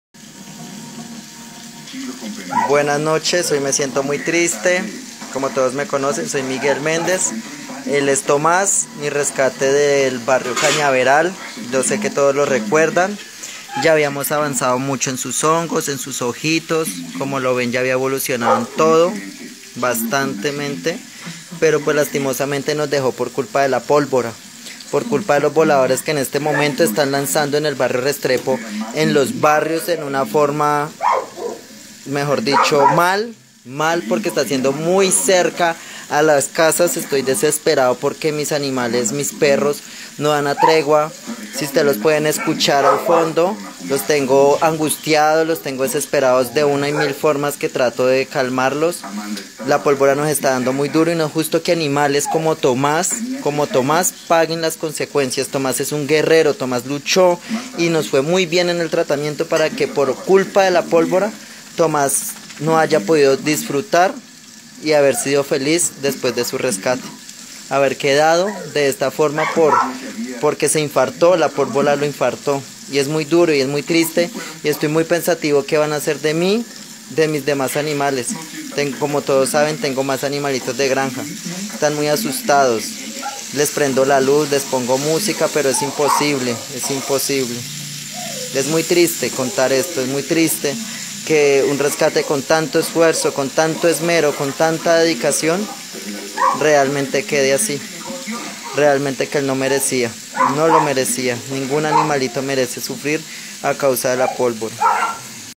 Quema de pólvora en el barrio Restrepo ocasionó la muerte de una mascota
Tomás, un conejo que había sido recogido de la calle, falleció por el uso indebido de pólvora.

👉La denuncia la realizo Miguel Méndez, quien habría rescatado a Tomás en pésimas condiciones en el barrio Cañaveral.

⭕Según él, Tomás sufría de hongos y presentaba un problema en sus ojos que, con el tiempo y mucho esfuerzo, Méndez pudo estabilizar.

🤨Sin embargo, la noche del 4 de diciembre en el barrio Restrepo, algunos vecinos arrojaron voladores muy cerca a su vivienda, ocasionando que el conejo sufriera desespero, arritmia cardiaca, estrés, desorientación y, por último, un infarto.